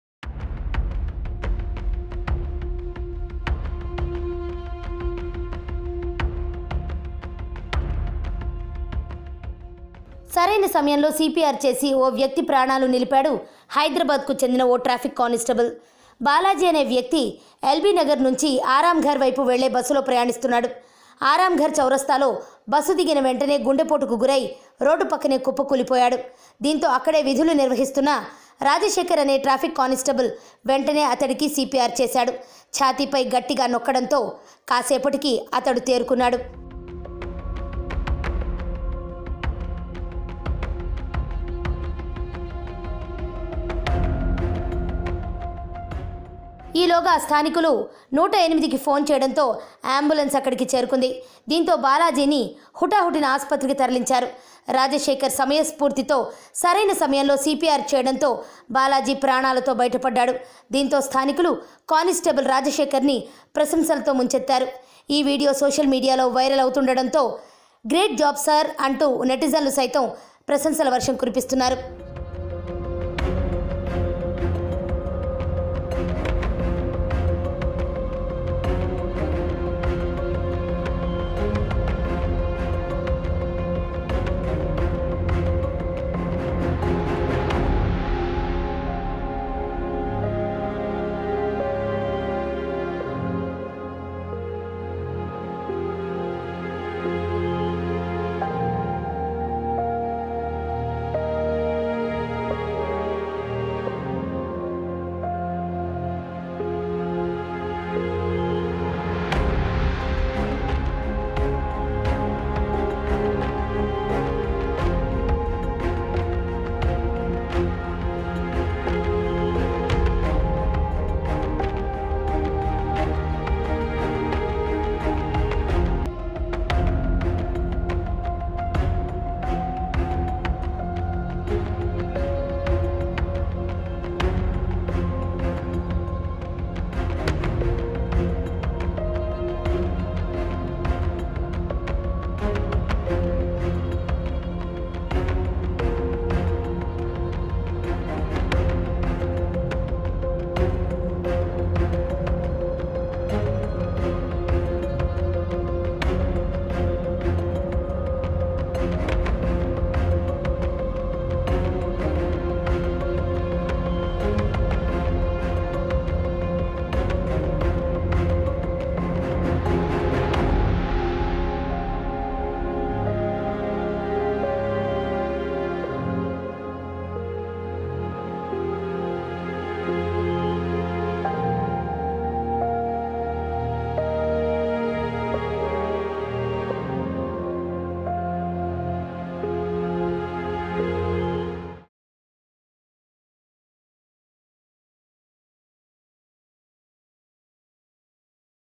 Traffic Cop Saves Life of Man in Telangana | CPR | Rajendranagar | Samayam Telangana
Traffic Cop Saves Life of Man in Telangana.